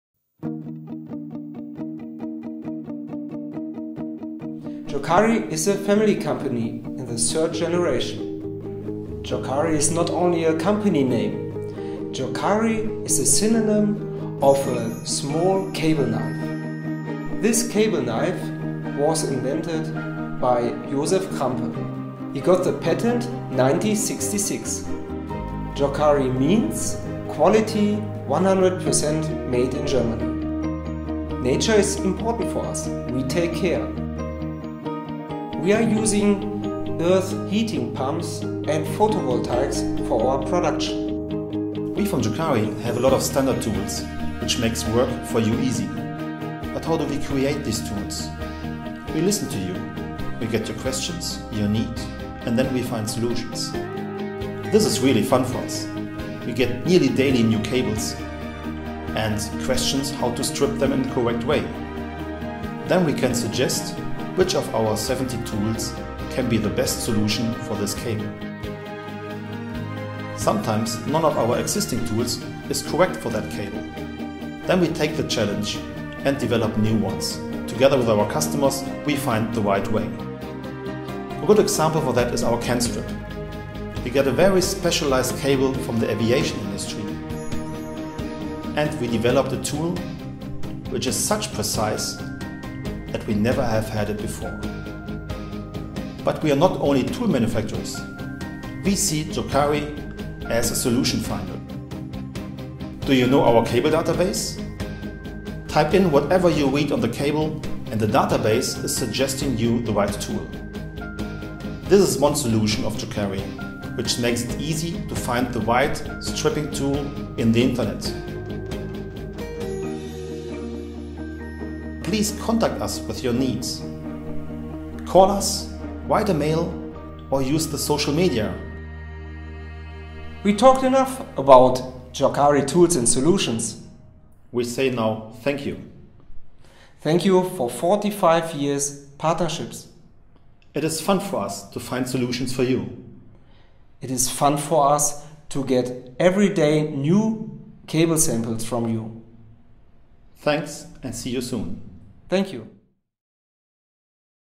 Jokari Cable Stripping Tools
Overview of Jokari, the market leader in design and manufacture of Cable Stripping Tools and partner of Demesne Electrical.
Full range available here with express delivery guaranteed...